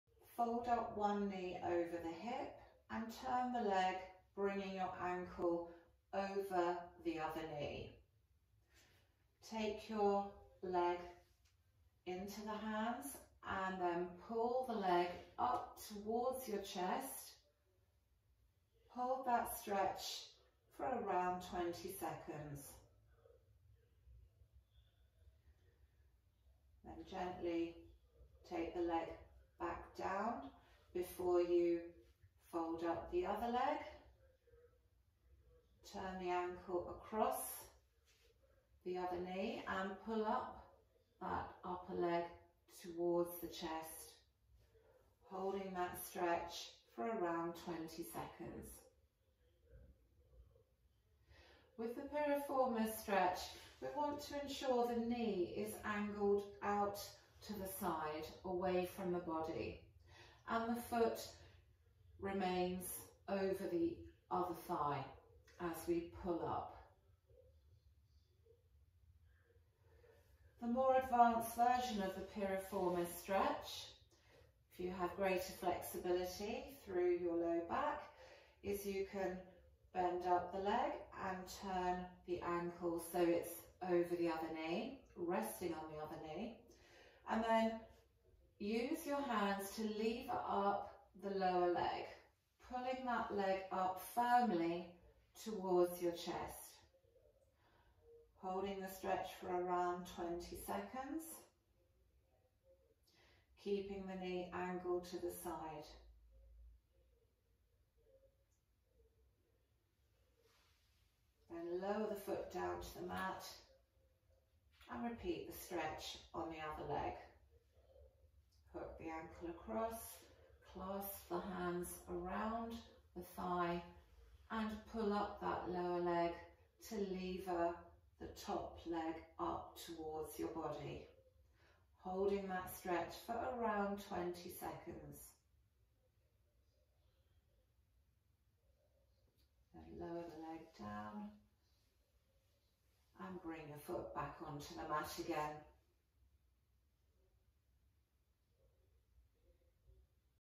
Buttock / Piriformis Stretch Lying Down- by Gina John Osteopath & Pilates Trainer
Buttock Piriformis Muscle Stretch by Gina John - Osteopath & Pilates Trainer

This exercise is often prescribed for patients with back,  hip and knee pain,  as well as conditions such as Trochanteric Bursitis, and Sciatica.

It is important to discontinue any exercise which causes pain.

Or
The_osprey_clinic
Or